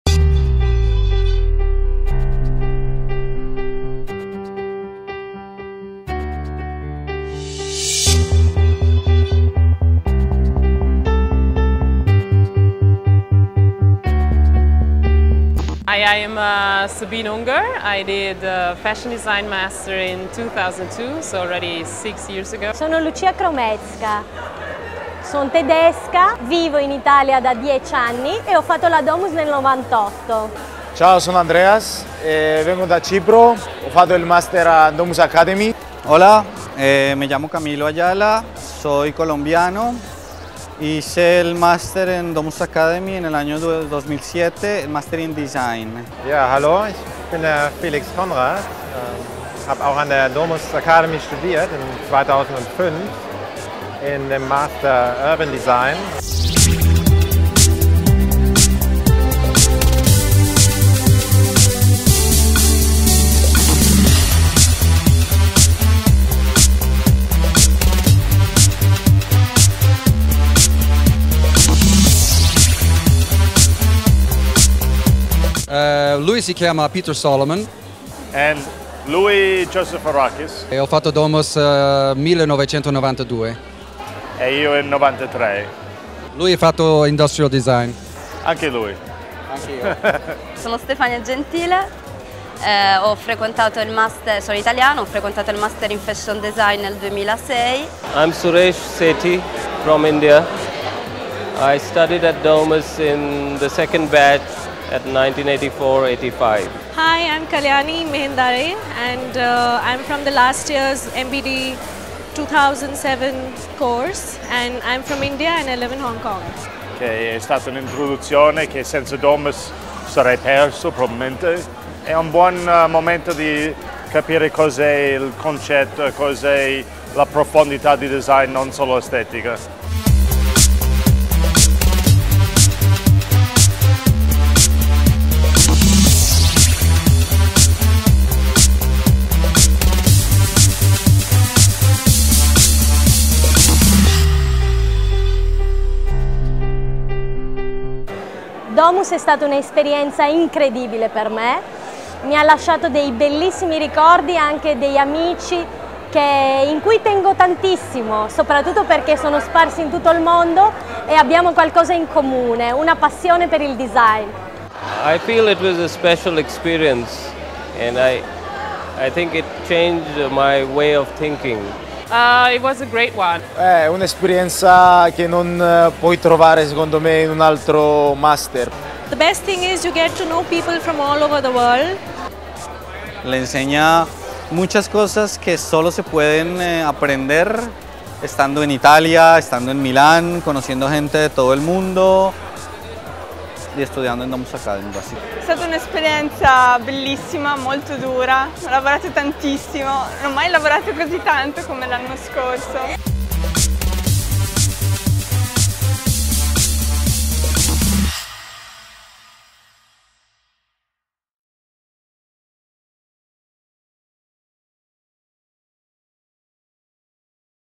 Domus Academy Alumni Reunion 2008
Interviews with Alumni during the reunion of 2008 at That's Design during the Fuori Salone